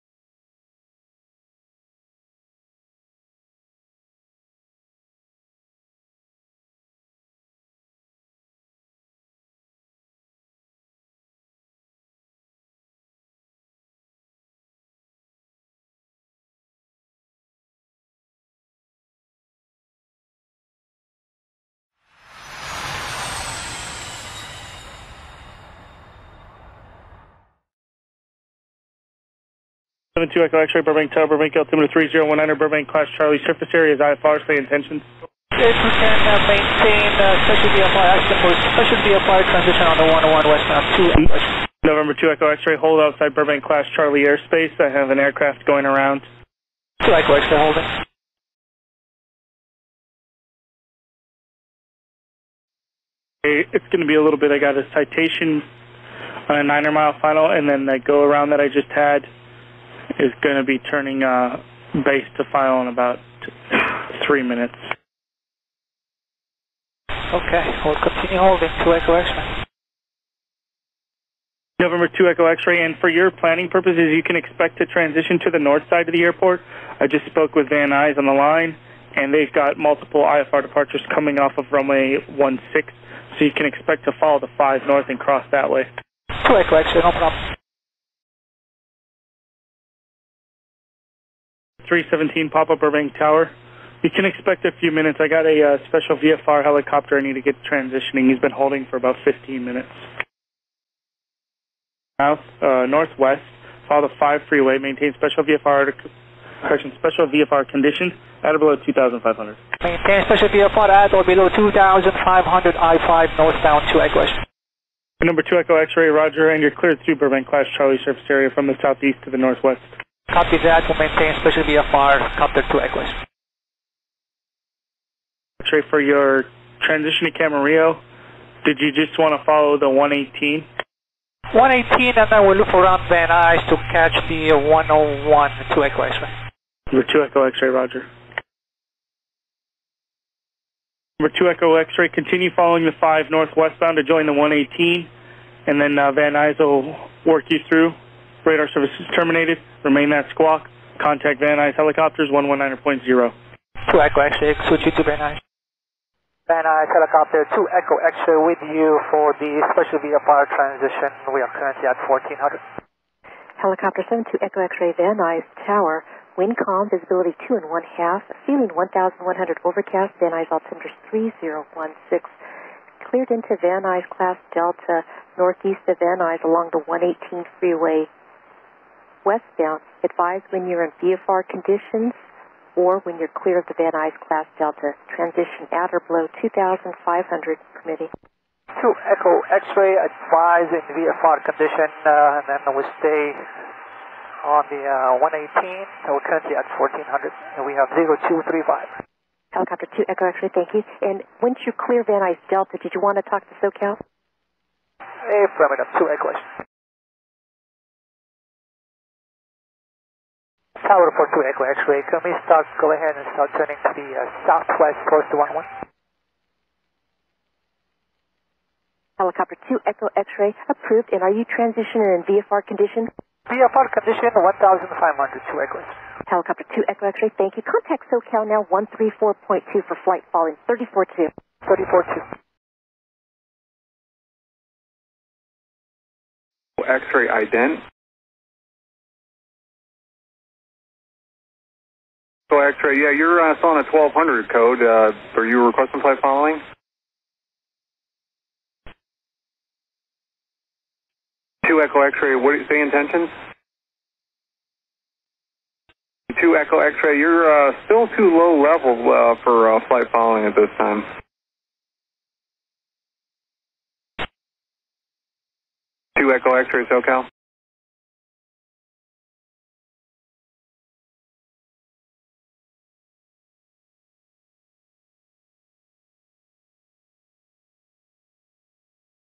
Kobe Bryant Helicopter Crash - Pilot to Air Control Interaction - SIKORSKY CRASH - VFR WEATHER
Last interaction between Kobe’s pilot and air traffic control.